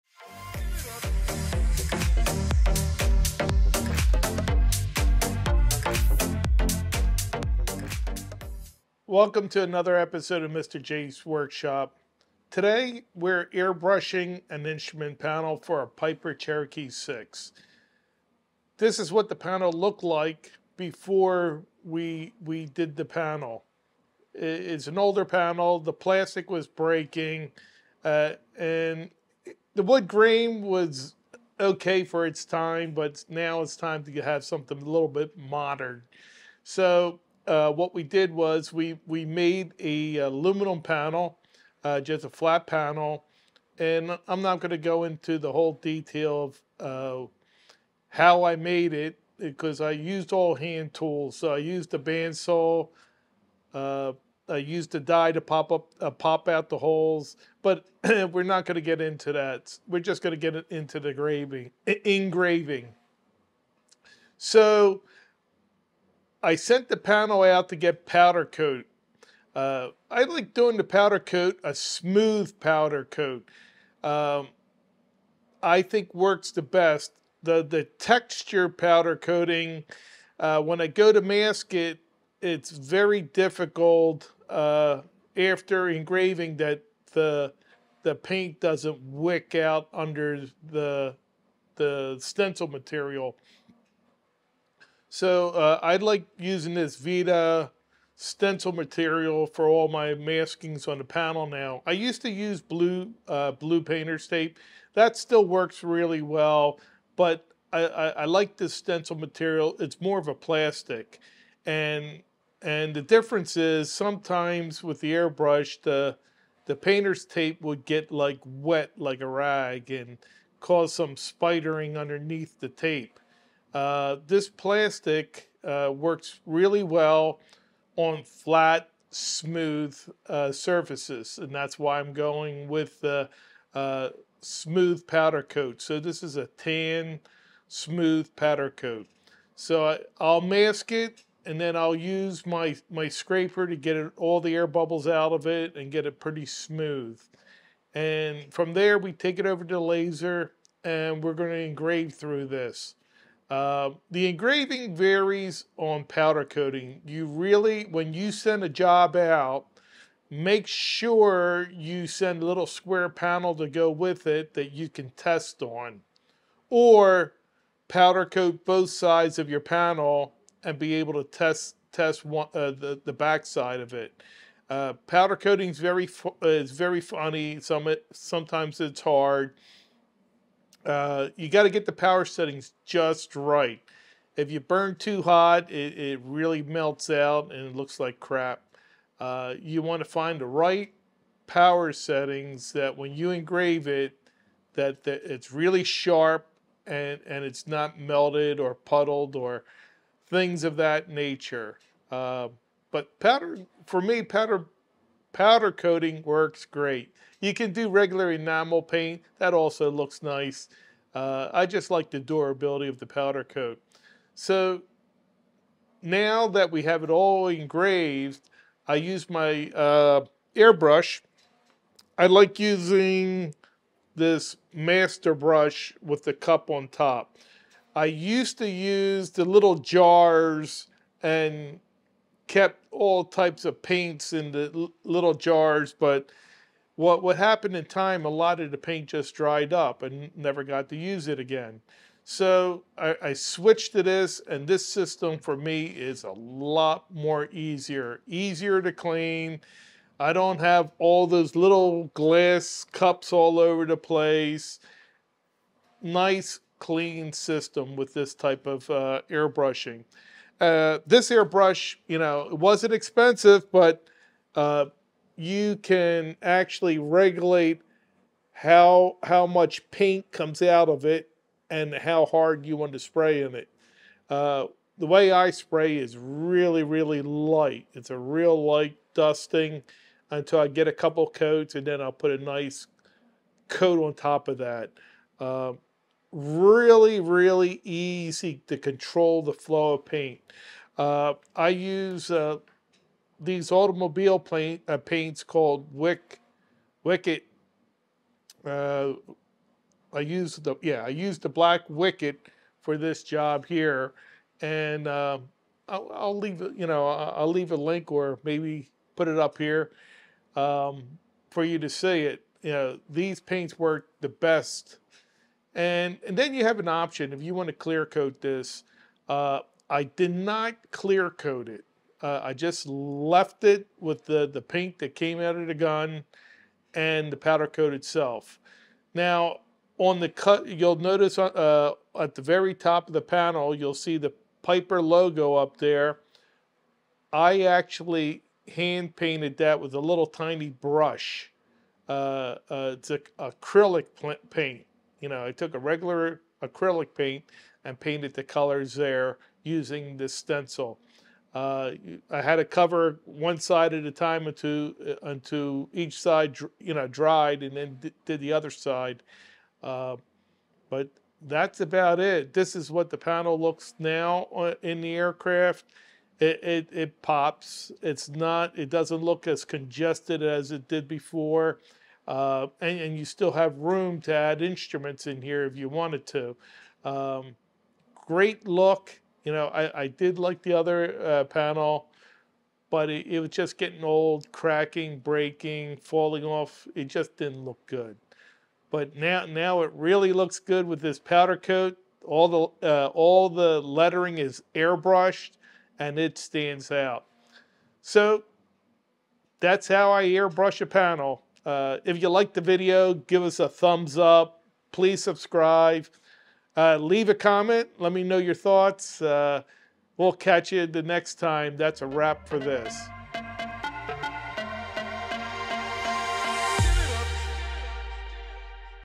TIP & TRICKS Laser engraving and airbrush aircraft panel
MR. J's WORKSHOP

MATERIAL USE
VViViD Blue Stencil Vinyl Masking Film with Anti-Bleed Technology (25ft x 12in)
Airbrush: MASTER
Scraper: plastic scraper
Paint: WICKED

TOOLS USED: Epilog Fusion 50w Laser, CorelDRAW